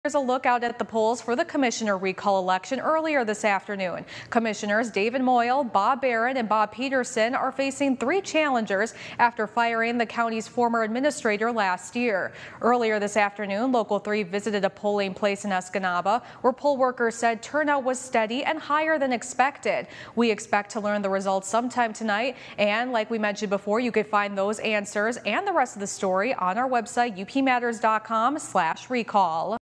Your Local Election Headquarters: Delta County Recall Election
Here's a look out at the polls for the commissioner recall election earlier this afternoon.